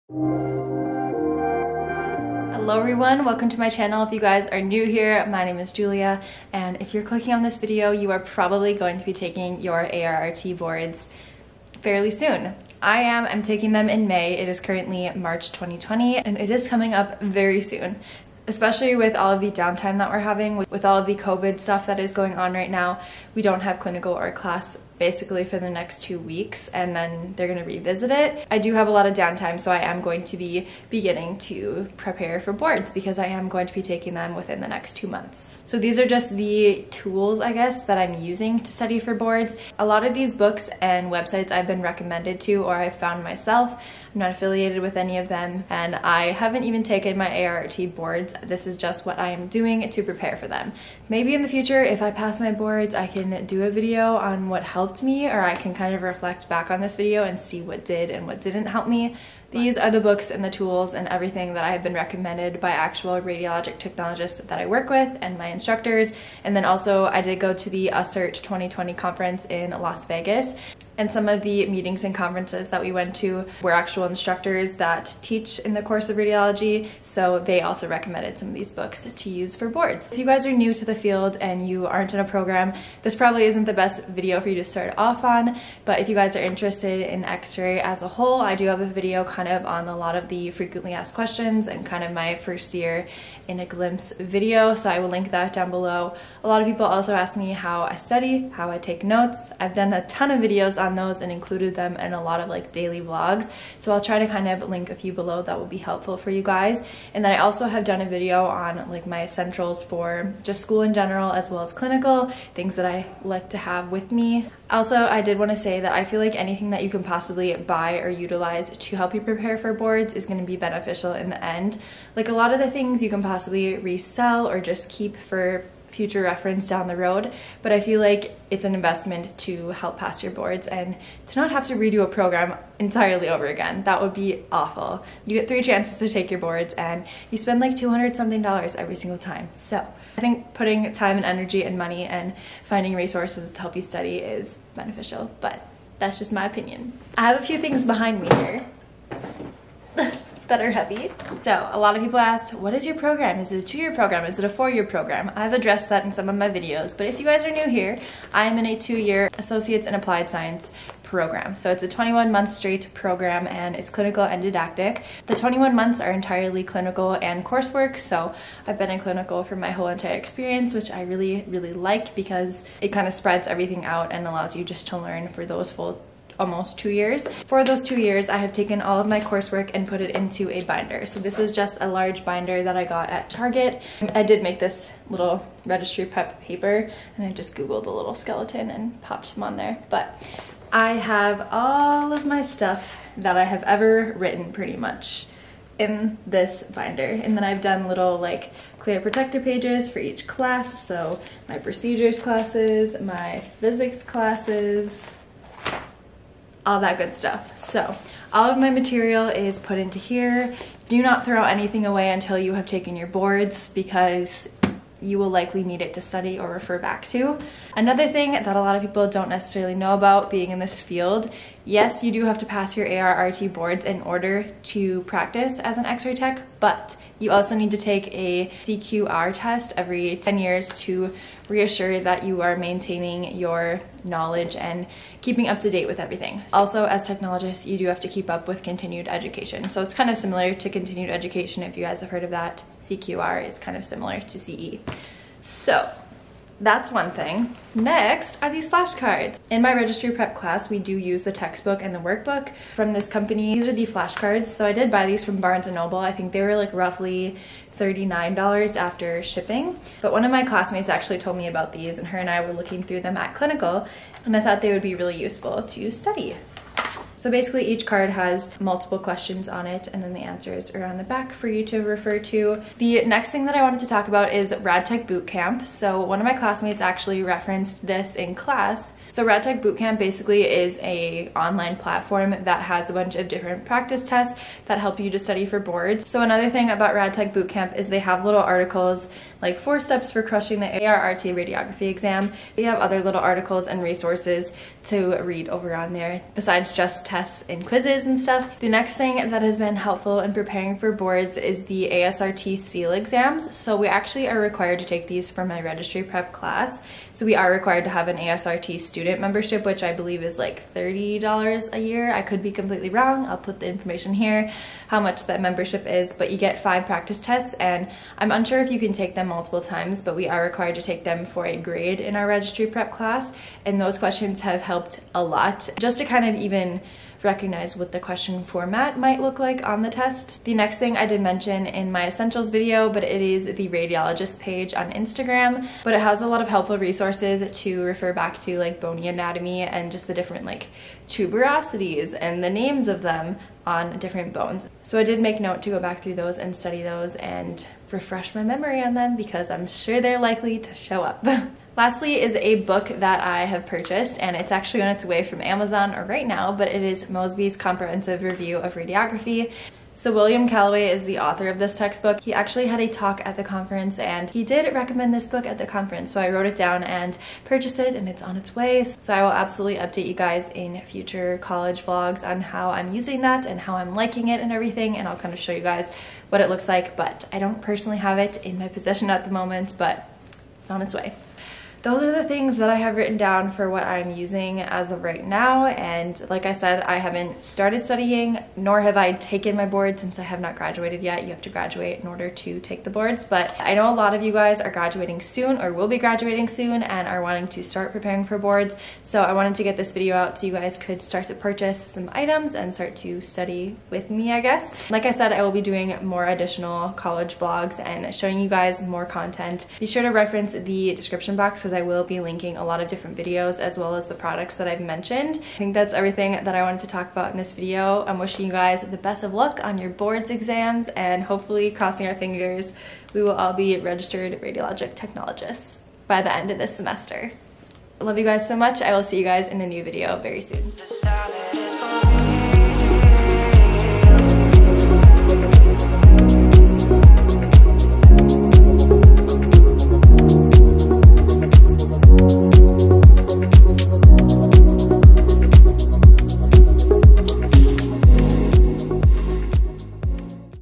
things I’m using to study + prep for radiology registry exam
✧ here are the materials I am using to help study for my radiology registry exam! everything I mentioned is linked below!

Mosby's comprehensive review book

⋒ x r a y   s c h o o l   v i d e o s

age ⋆ 23
location ⋆ North Dakota
camera ⋆ Canon G7x Mark II + Canon M50